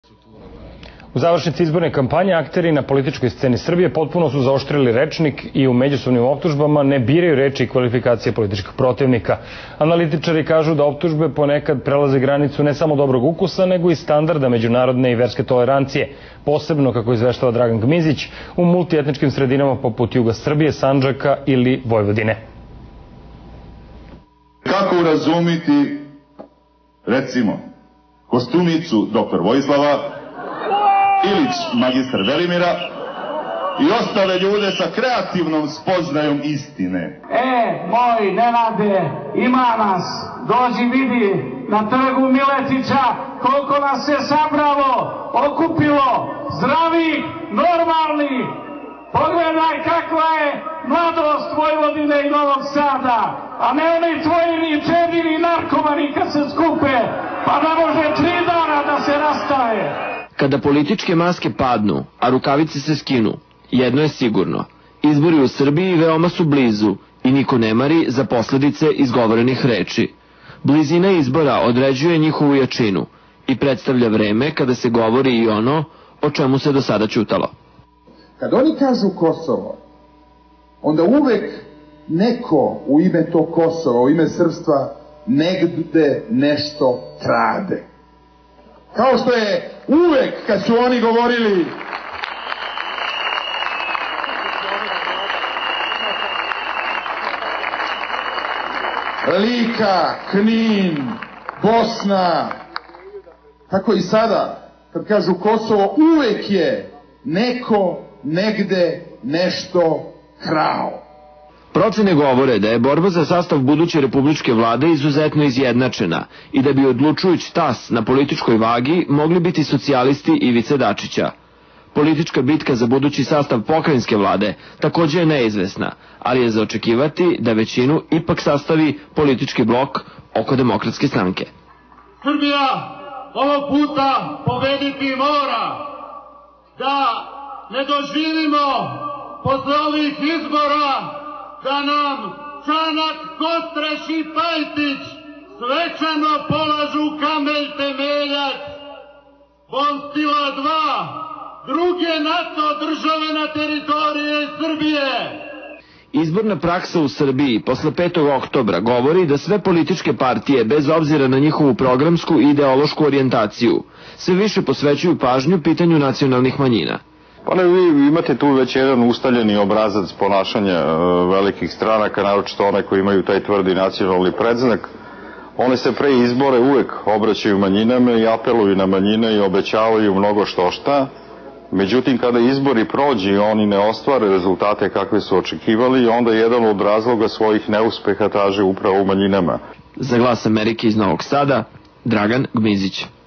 Zaoštren rečnik u završnici izborne kampanje u Srbiji
U završnici izborne kampanje akteri na političkoj sceni Srbije potpuno su zaoštrili rečnik i u medjusobnoj borbi ne biraju reči kvalifikacije politickih protivnika.